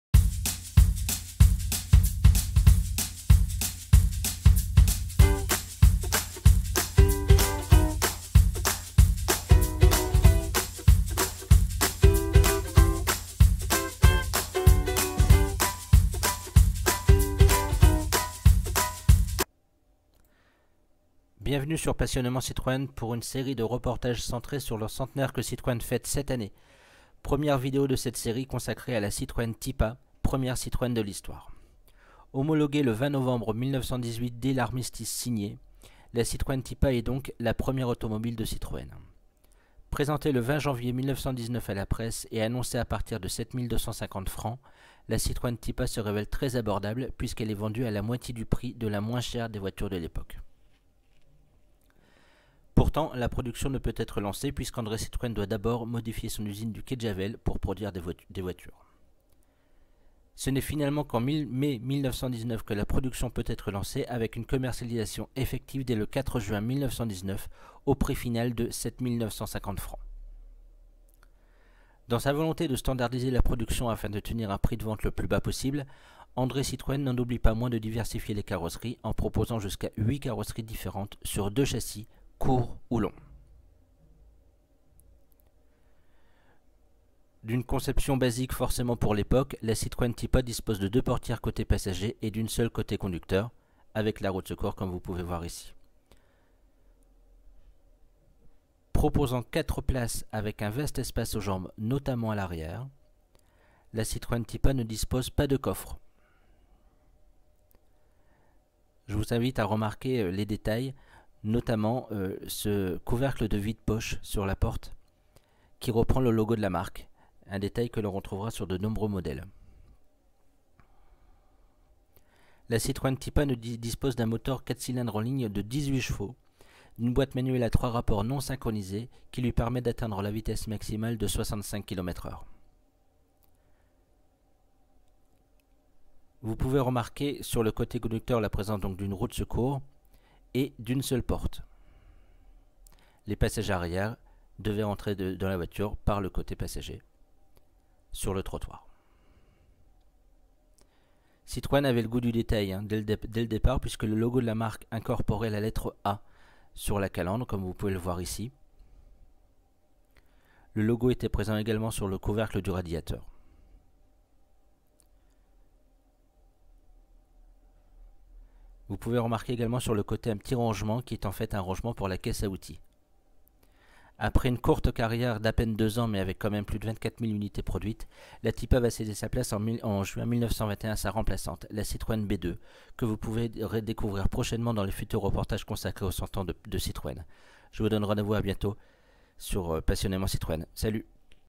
Centenaire Citroën : La Citroën Type A
Spéciale centenaire de Citroën : première vidéo de cette série de reportages sur 52 véhicules mythiques de Citroën.  La Type A est sortie en 1919 et est donc la toute première voiture de Citroën